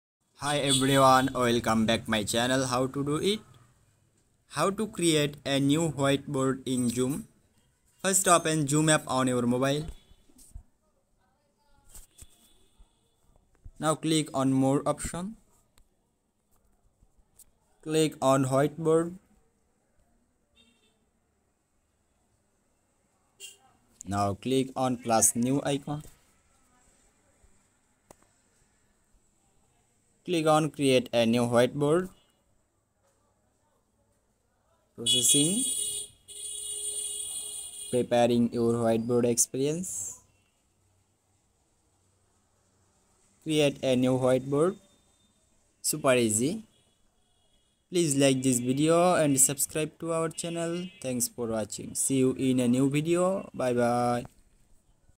How to create a new whiteboard in zoom 2024 | Skill Wave
Hello, My Dear Friends, !! In this video, I will show you How to create a new whiteboard in Zoom. Watch the video till the end Thanks.

Step 1: Sign in to the Zoom web portal.
Step 2: Click Whiteboards.
Step 3: At the top right, click + New to create a new whiteboard.

Chapters:
0:00  Introduction
0:08 Create a new whiteboard on Zoom
0:47  Happy Ending